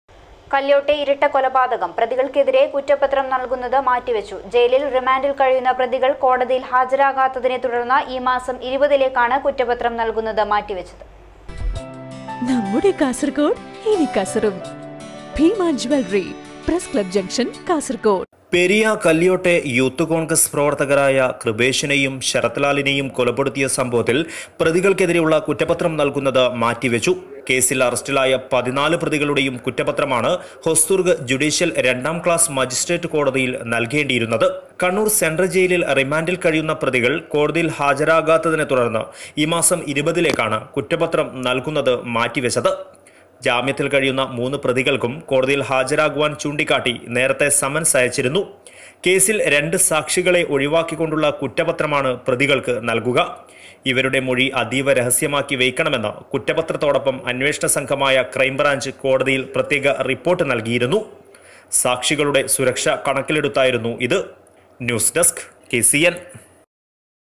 കല്യോട്ടെ ഇരട്ട കൊലപാതകം. പ്രതികള്‍ക്കെതിരെ കുറ്റപത്രം നല്‍കുന്നത് മാറ്റിവെച്ചു
കല്യോട്ടെ ഇരട്ട കൊലപാതകം. പ്രതികള്‍ക്കെതിരെ കുറ്റപത്രം നല്‍കുന്നത് മാറ്റിവെച്ചു. ജയിലില്‍ റിമാന്‍ഡില്‍ കഴിയുന്ന പ്രതികള്‍ കോടതിയില്‍ ഹാജരാകാത്തതിനെതുടര്‍ന്ന് ഈ മാസം 20 ലേക്കാണ്  കുറ്റപത്രം നല്‍കുന്നത് മാറ്റിവെച്ചത്.